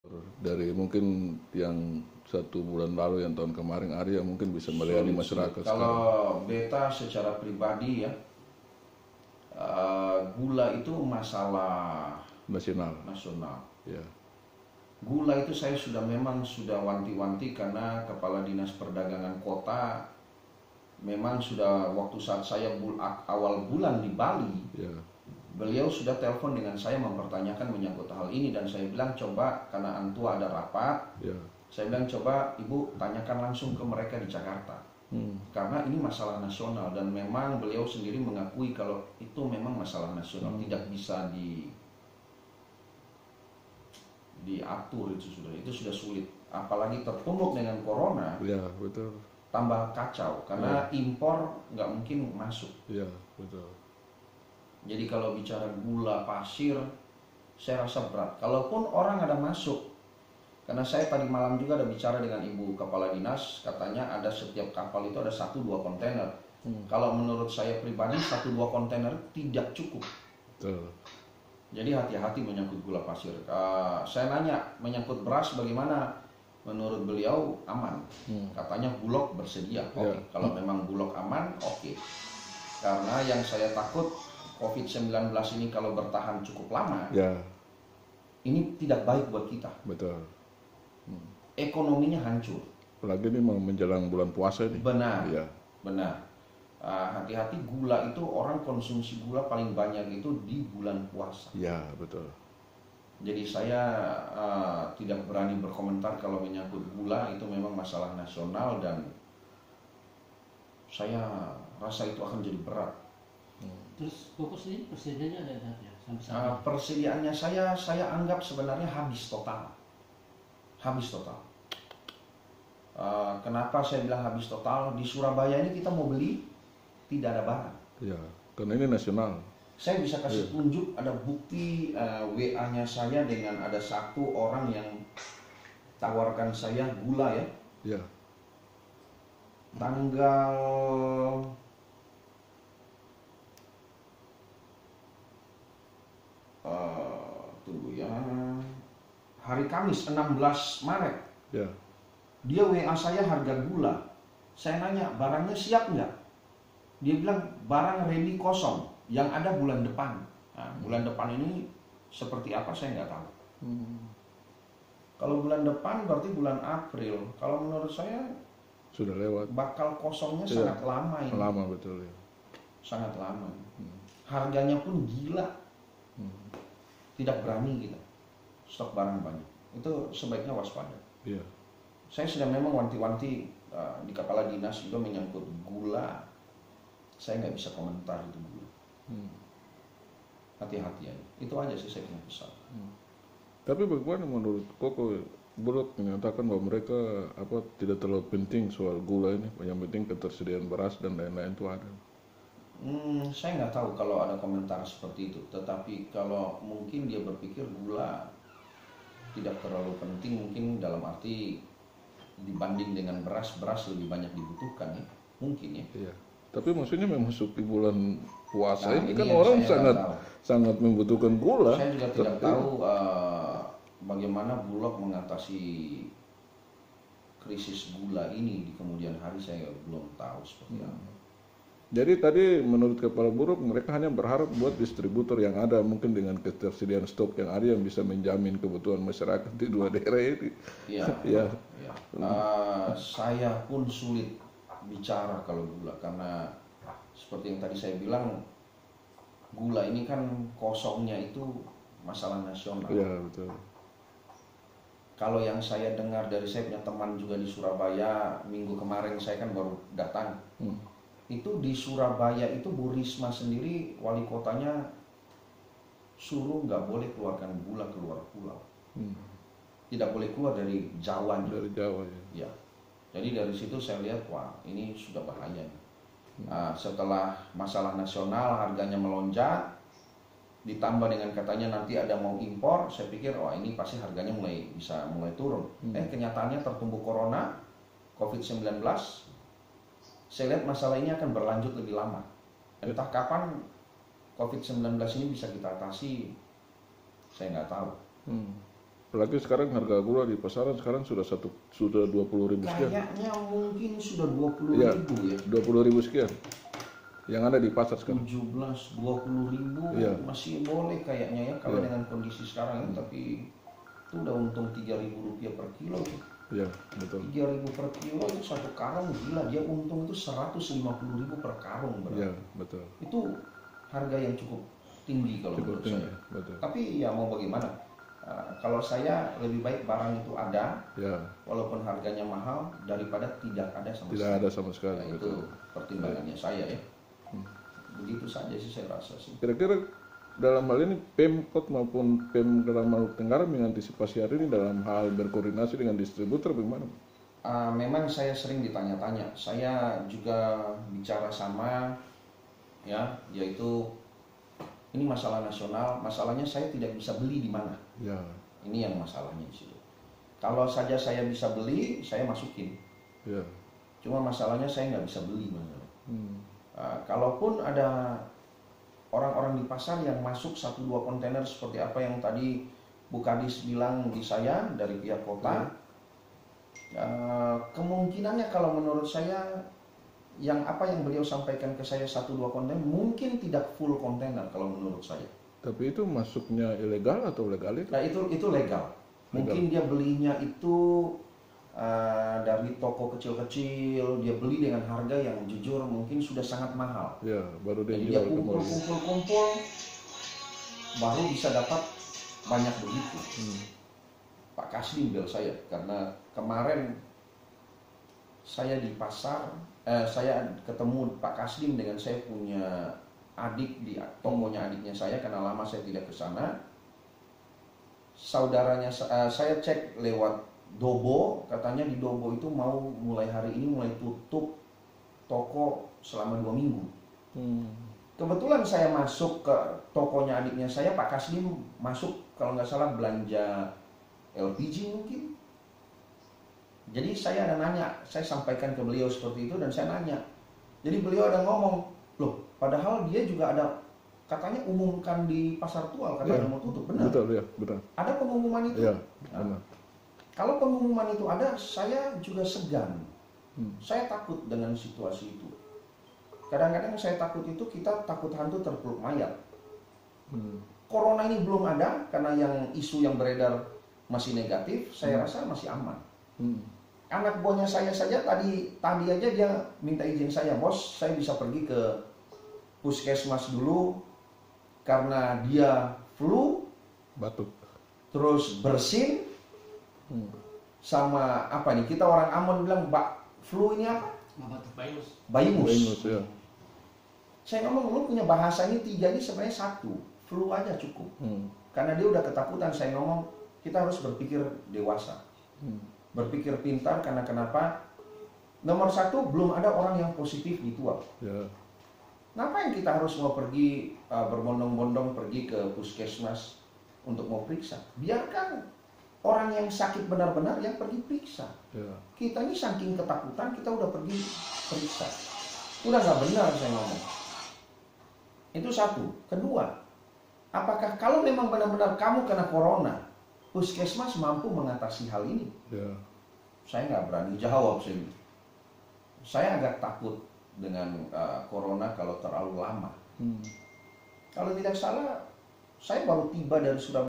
DISTRIBUTOR MENYERAH, KRISIS GULA PASIR DI PULAU KEI BAKAL LAMA
Krisis gula pasir di Pulau Kei.Propinsi Maluku